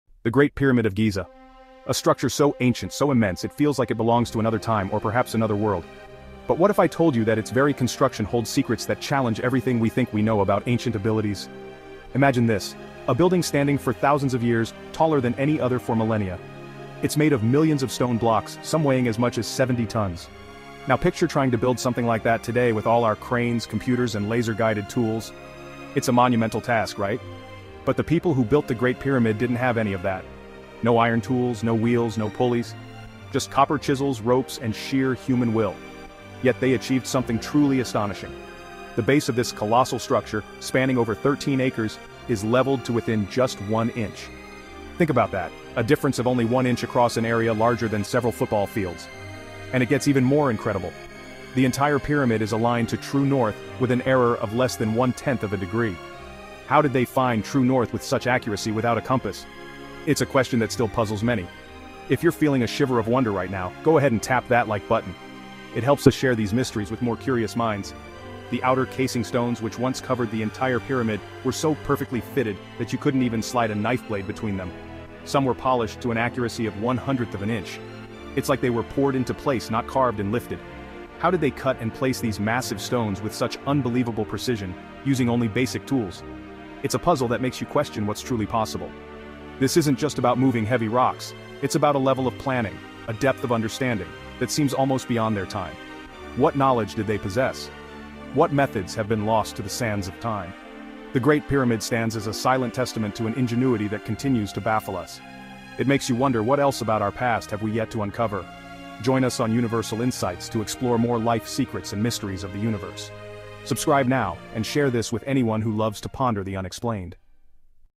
70-Ton Blocks & Knife-Edge Gaps: How Did Giza's Builders Do It?
Step into the ancient world and uncover the mind-bending engineering behind the Great Pyramid of Giza! This YouTube Short explores the incredible precision and baffling construction methods used by ancient Egyptians, leaving us to wonder how they achieved such monumental feats without modern tools.

Discover astonishing facts:
How were millions of stone blocks, some weighing up to 70 tons, moved and placed?
What allowed the pyramid's massive base, spanning over 13 acres, to be leveled to within just one inch?
How did they align the entire structure to true north with an error of less than one-tenth of a degree, without a compass?
The mystery of the outer casing stones, fitted so perfectly you couldn't slide a knife blade between them.

Join us as we delve into the secrets of this ancient wonder, challenging our understanding of historical capabilities and sparking curiosity about the ingenuity of our ancestors.

Bibliography:
The information presented in this script is based on widely accepted archaeological and engineering research regarding the Great Pyramid of Giza. Specific facts and figures are supported by:
Stone Weight: Average core blocks around 1.5-2.5 tons, with some granite blocks in internal chambers weighing up to 70-80 tons.egyptabout.com

Leveling Accuracy: The base of the Great Pyramid is leveled to within approximately one inch (or 2 cm) over its 13.1-acre area.upenn.edu

True North Alignment: The pyramid is aligned to true north with remarkable accuracy, deviating by less than one-tenth of a degree (or within 3-5 arcminutes).sciencealert.com

Casing Stone Precision: The outer casing stones were fitted with extreme precision, with joints as fine as 0.5 millimeters, making it impossible to insert a knife blade between them.wikipedia.org

References:
"Construction of the Egyptian pyramids - Wikipedia." Wikipedia,

Baldridge, Jason. "Moving and Lifting the Construction Blocks of the Great Pyramid." Moving and Lifting the Construction Blocks of the Great Pyramid, 1996,

"Great Pyramid of Giza - Wikipedia." Wikipedia,

ScienceAlert. "The Secret of The Pyramids' Perfect Alignment Might Be Explained After All." ScienceAlert, 7 Apr. 2022,

Quora. "How big is the Great Pyramid of Giza and how much does it weigh?" Quora, 12 Aug. 2023,

TheCollector. "The Great Pyramid of Giza: 5 Facts About the Architectural Marvel." TheCollector, 8 July 2023,

KidsAncientEgypt.com. "16 Fun Facts About The GREAT PYRAMID." KidsAncientEgypt.com,

Smithsonian Magazine. "Is the Fall Equinox the Secret to the Pyramids'

Near-Perfect Alignment?" Smithsonian Magazine, 22 Feb. 2018,

Quora. "Is the great pyramid aligned to true north?" Quora, 8 Aug. 2019,

World of Antiquity. "How the GREAT PYRAMID was Aligned to True North | ATG Highlights." YouTube, 3 Aug. 2023,

HowStuffWorks. "Building the Pyramids." Science | HowStuffWorks,